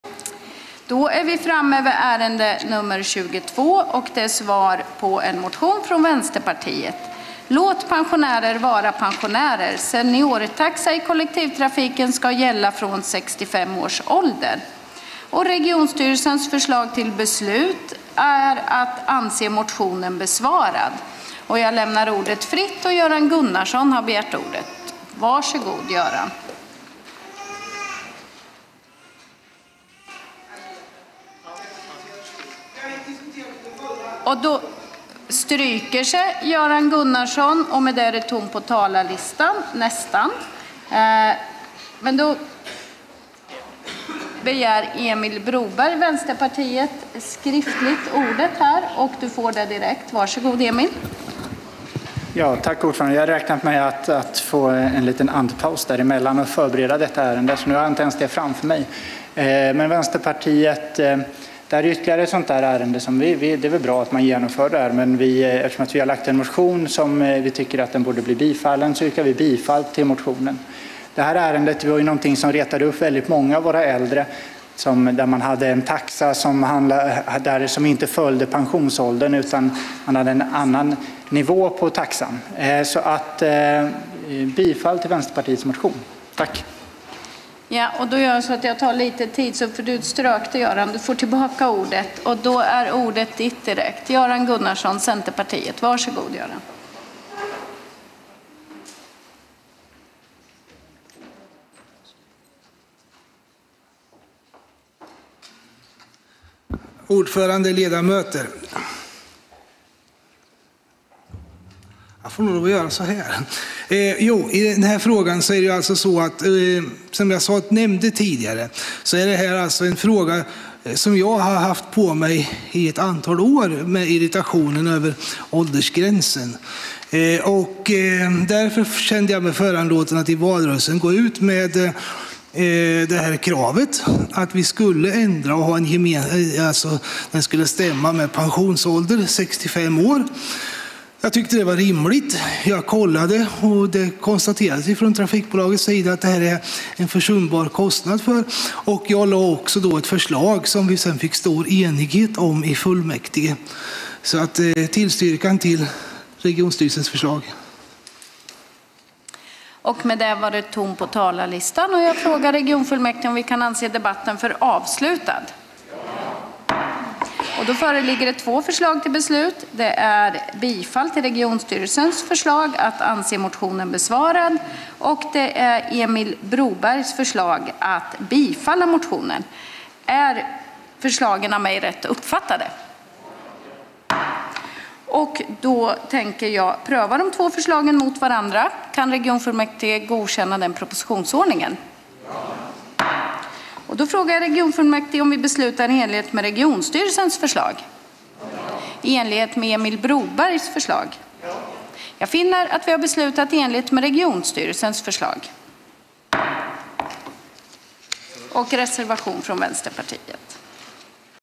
g) Ärende 22
Ärende 22 Svar på motion: Från Vänsterpartiet Låt pensionärer vara pensionärer, seniortaxa i kollektivtrafiken ska gälla från 65 års ålder